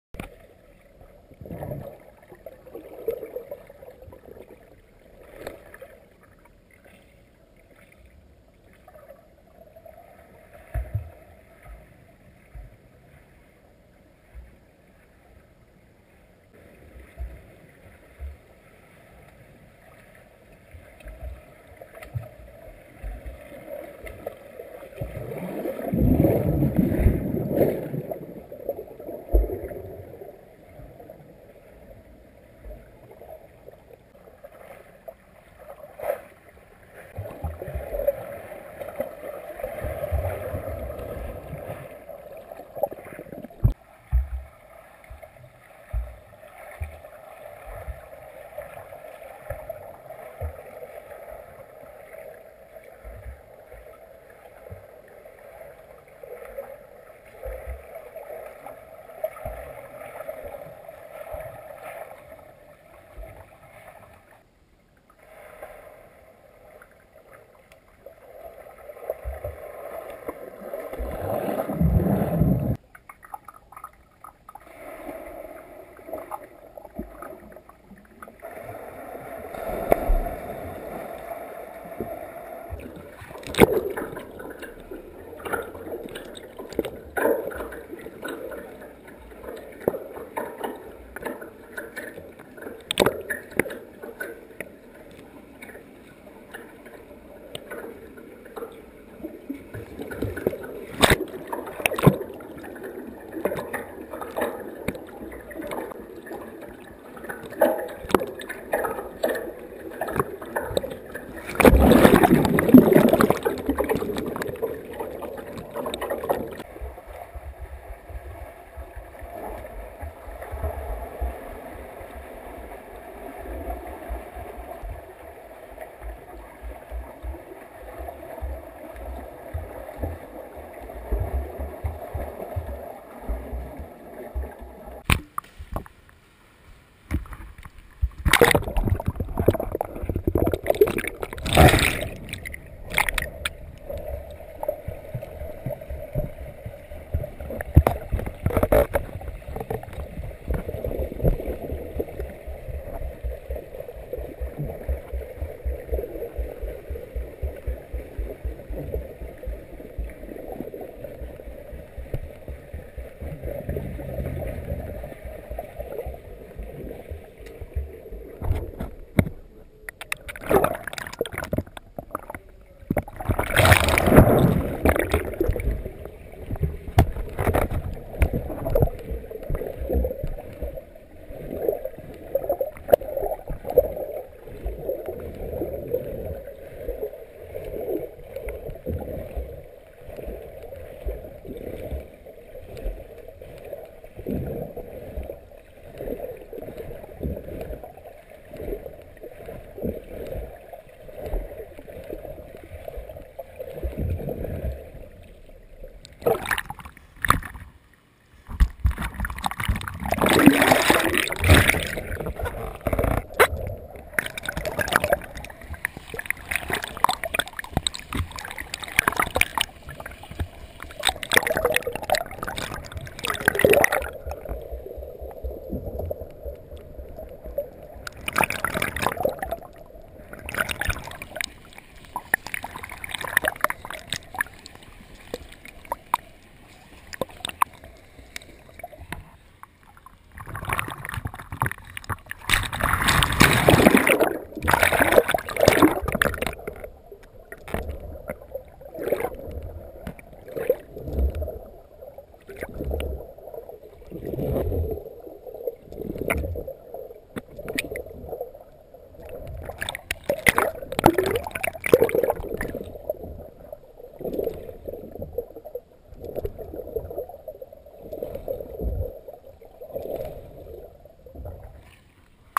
Pool Shots
I tested out a variety of perspectives in the pool.